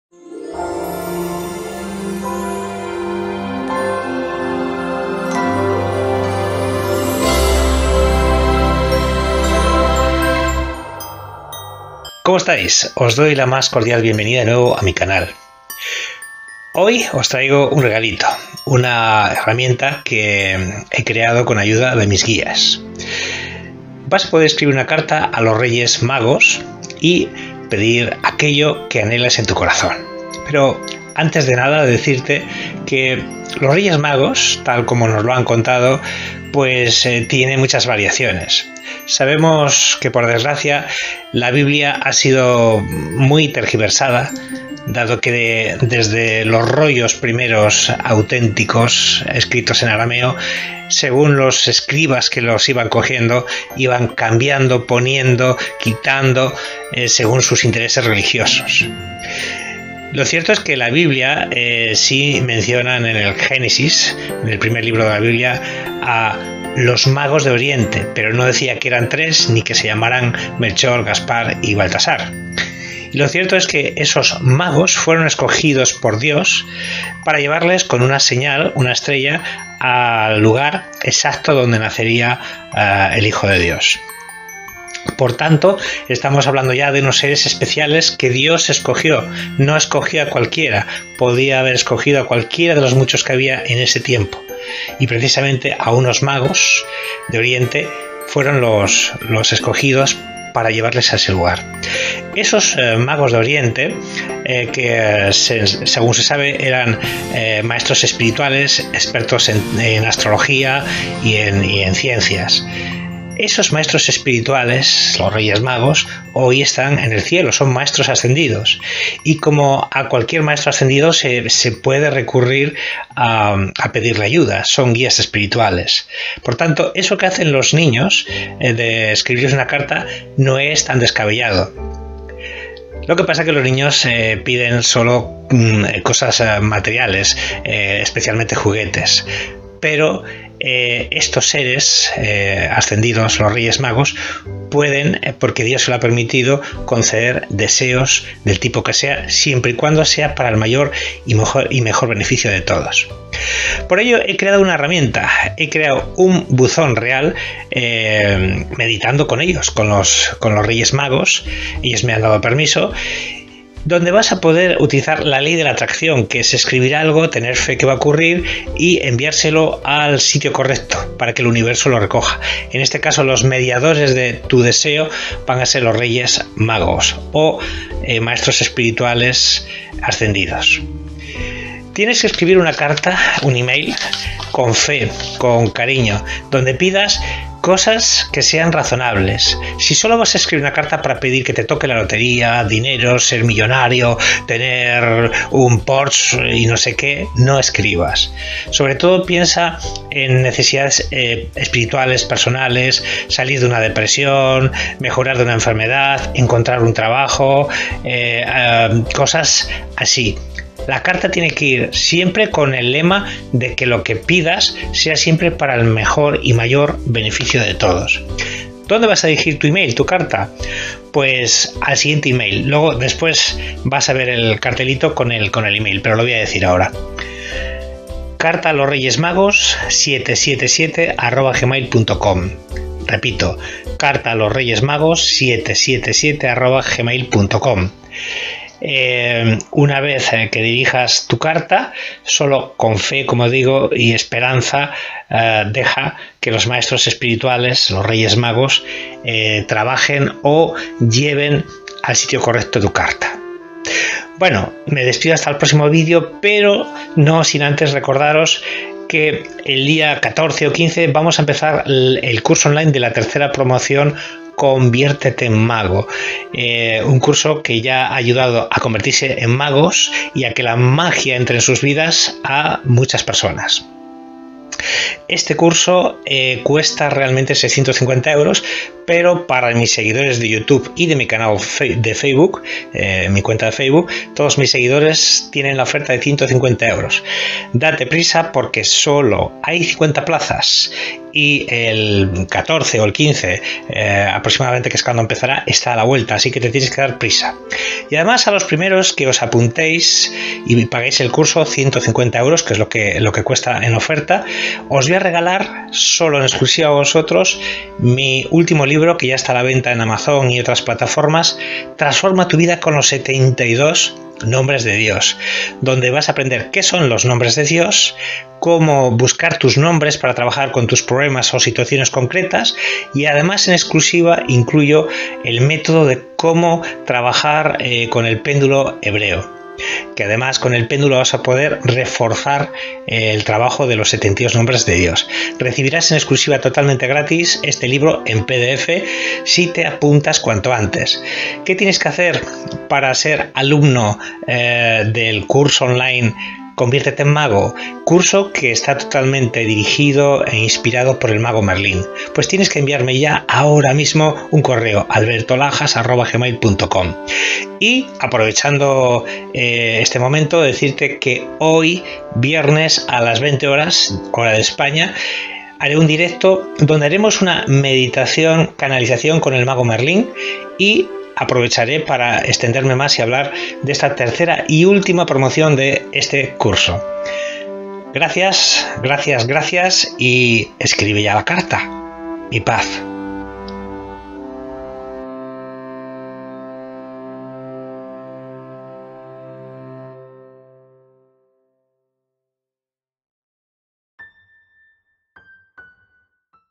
ESCRIBE A LOS REYES MAGOS
Ahora puedes escribir una carta a los magos de oriente y pedir que se cumplan tus deseos...